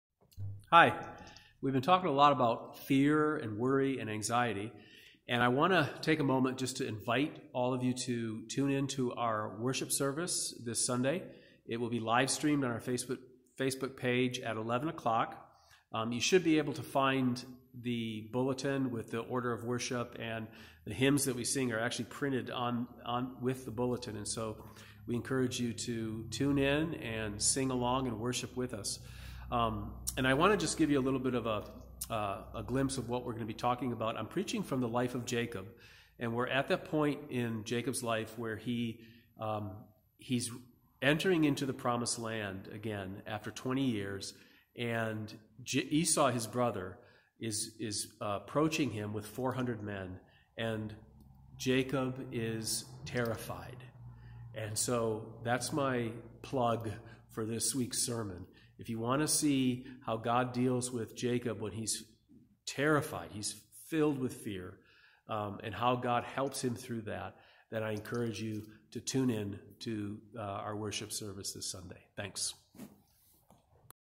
Invitation to worship
Pastor Belden invites you to worship with us this Sunday as we live stream our service on our Facebook